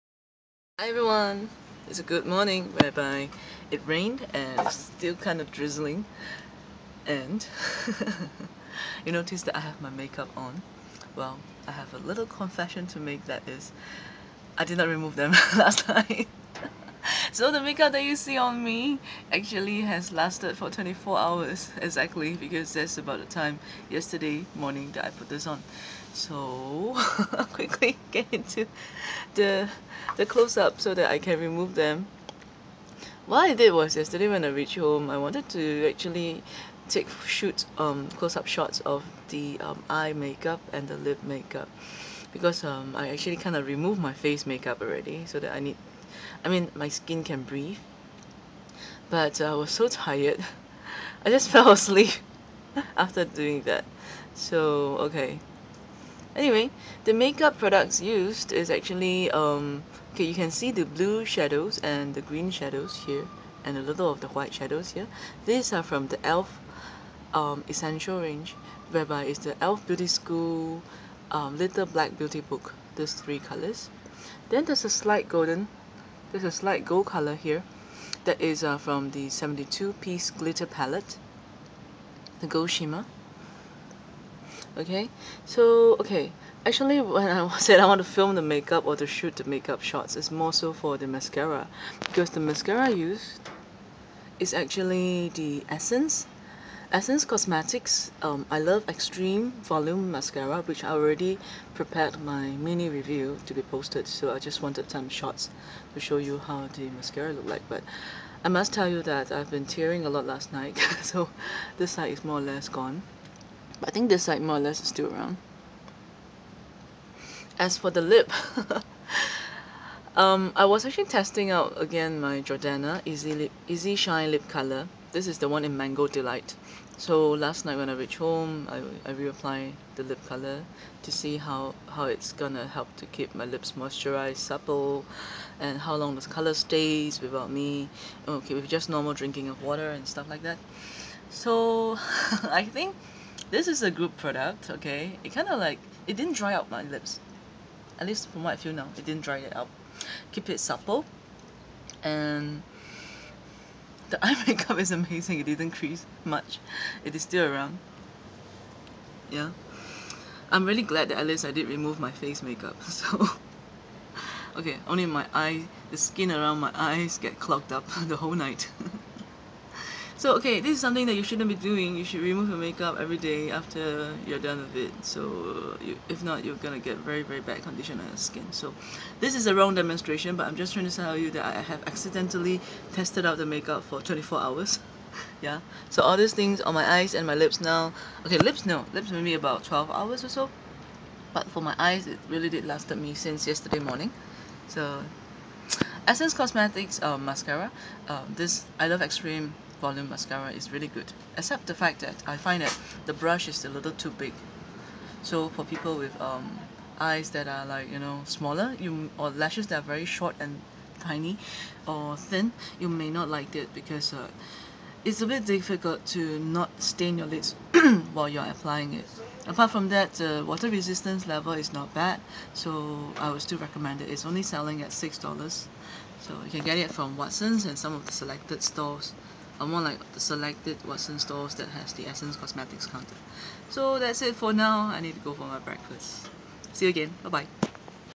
[Mini Review] essence I LOVE EXTREME Volume Mascara (incl. elf & Jordana products too)
This is one of the rare occasions where I forgot to remove my eye n lip makeup and set the products through a 24hrs + sleep-in test.

Not a right thing to do..